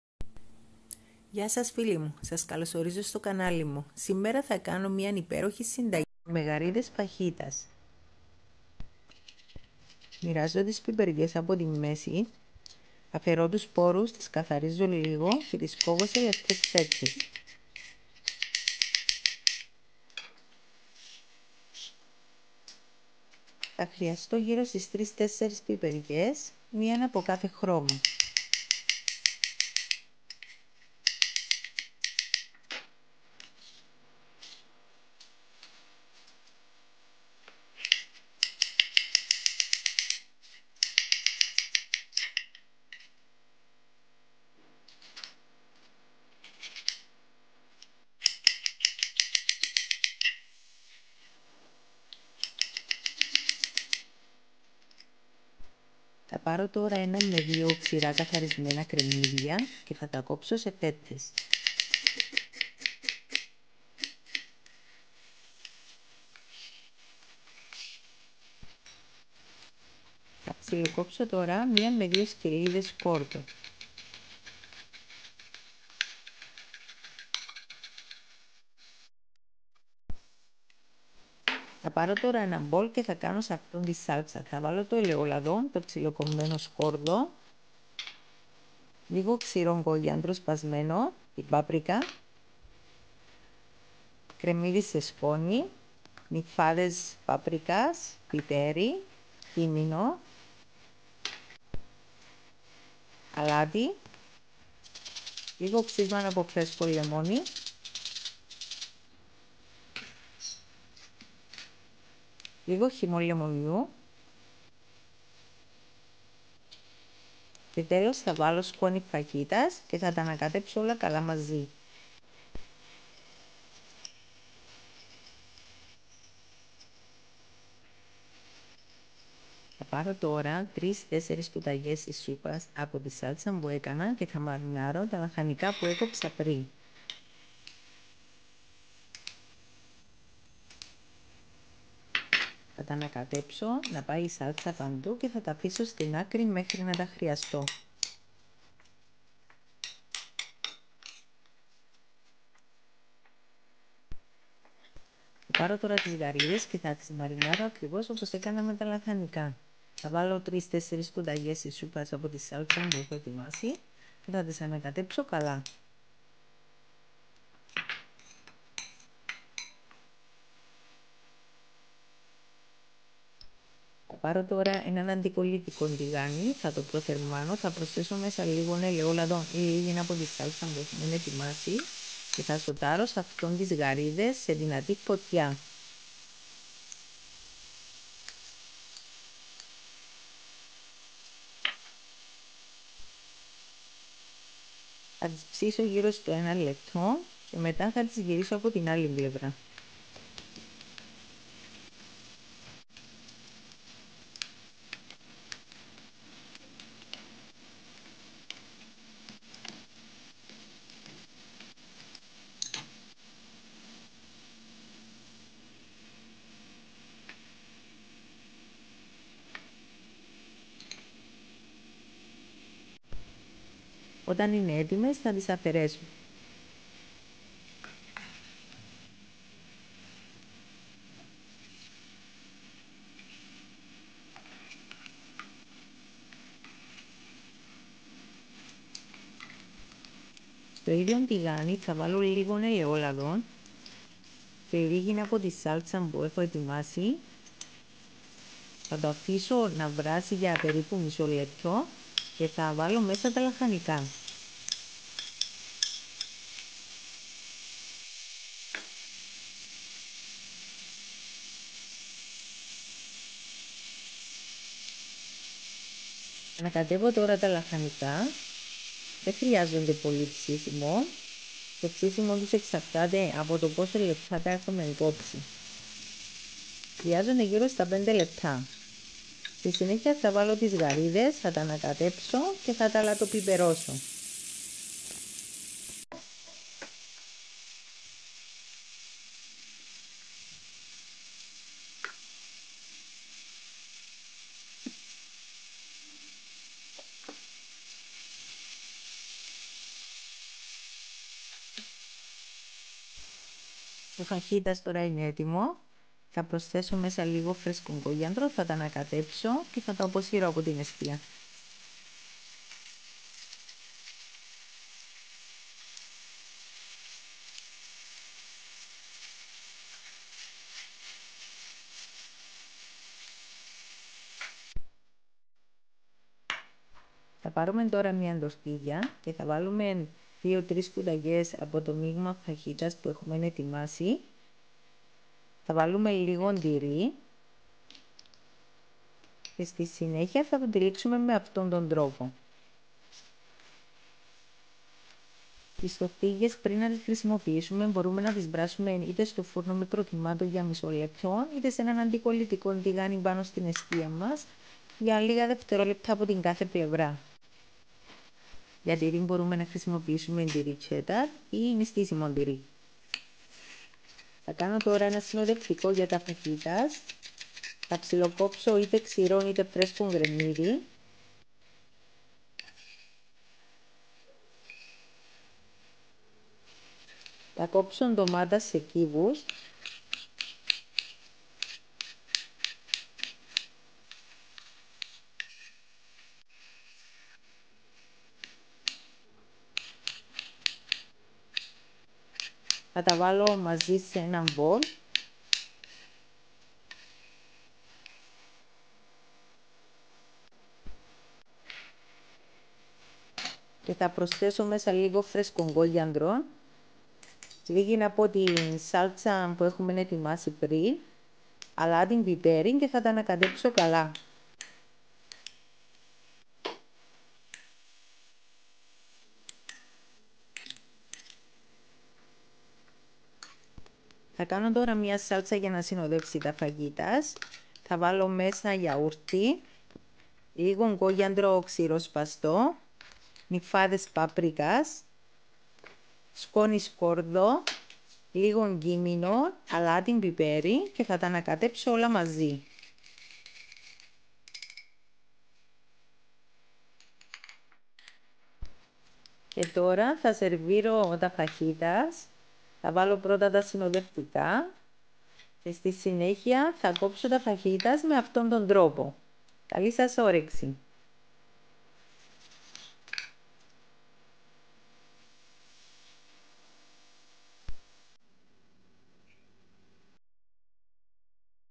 Φαχιτας με Γαρίδες Νηστίσιμο!!!
Υπέροχο Φαχιτας με Γαρίδες.
Δεν υπάρχει λόγος να στερήσε τα αγαπημένα σου φαγητά στην νηστεία!

Υλικά :
4 πιπεριές (μια απο κάθε χρώμα).

2 ξηρά κρεμμύδια.

600 γρ. γαρίδες καθαρισμένες.

Για την σάλτσα Φαχιτας.

1/2 ποτήρι ελαιόλαδο.

2 σκελίδες σκόρδο ψιλοκομμένο.

Ξύσμα απο ένα λεμόνι.

2-3  κουταλιές της σούπας χυμό λεμόνι.

1 κ. σ. Ξηρό κολιανδρο σπασμένο.

1 κ. γ κύμινο.

1 κ. γ. Πάπρικα σκόνη.

1 κ. γ. Νιφάδες πάπρικας.

1 κ. σ. Σκόνη φαχιτας.

1 κ. σ. Σκόνη κρεμμύδι.

Αλάτι.

Πιπέρι.
Ανακατευω όλα τα υλικά μαζι.

Για σερβίρισμα :

Λίγο φρέσκο κολιανδρο.

4 πίτες  τορτιγιες.

Για το συνοδευτικό με την ντομάτα.
2 ντομάτες.

1 φρέσκο κρεμμυδάκι.

Λίγο φρέσκο κολιαντρο ψιλοκομμένο.

3 - 4 κ. σ. Σάλτσα φαχιτας (που εχουμε ετοιμάσει).

Αλάτι και πιπέρι.

Ψιλοκόβω το κρεμμυδάκι , την ντομάτα σε κύβους και τα βάζω όλα σε ένα μπολ μαζί με τα υπόλοιπα υλικά και ανακατεύω.

Συνοδευτική σάλτσα γιαουρτιού :

3-4  κ. σ. Γιαούρτι στραγγιστό.

 1/2 κ. γ. Σκόρδο σκόνη.

1/2 κ. γ. Κύμινο.

1/2 κ.γ κολιαντρο ξηρό σπασμένο.

 1/2 κ. γ. Κρεμμύδι σκόνη.

1/2κ.γ νιφαδες παπρικα.

1 κ. γ σκόνη φαχιτας.

Αλάτι πιπέρι.

Ανακατεύω όλα τα υλικά μαζί σε ένα μπολ.
Εκτέλεση συνταγής :

Μαρινάρω τα λαχανικά και τις γαρίδες με 3-4 κουταλιές απο την σάλτσα φαχιτας που ετοίμασα και τα ανακατεύω να πάει παντού.
Σοτάρω τις γαρίδες με 1 κ. σ. Ελαιόλαδο ελαιόλαδο  και 1-2 κ.σ σάλτσα φαχιτας, ένα λεπτό απο την κάθε πλευρά και τις αφαιρώ.
Στο ιδιο τηγάνι προσθέτω ξανά ελαιόλαδο και λίγη σαλτσα φαχιτας και σοτάρω τα Λαχανικά για 4-5 λεπτά.
Μολις είναι έτοιμα βάζω πίσω τις γαρίδες βάζω αλατοπίπερο  και λίγο κολιαντρο φρέσκο ανακατεύω και αποσύρω απο την εστία.
 Βράζω λίγο τις τορτιγιες σε ένα τηγάνι λίγα δευτερόλεπτα κάθε πλευρά. Η μισό λεπτό περίπου στο φούρνο μικροκυμάτων.

Σε μία τορτιγια βάζω 3-4 κ.σ γέμιση, λίγο τυρί και τυλίγω. Μπορώ να την βάλω 5 λεπτά στον φούρνο να ζεσταθεί  να λιώσει το τυρί και τη σερβίρω 🥰❤️
Την συνοδεύω με τα συνοδευτικά που έφτιαξα.
Καλη σας όρεξη!!!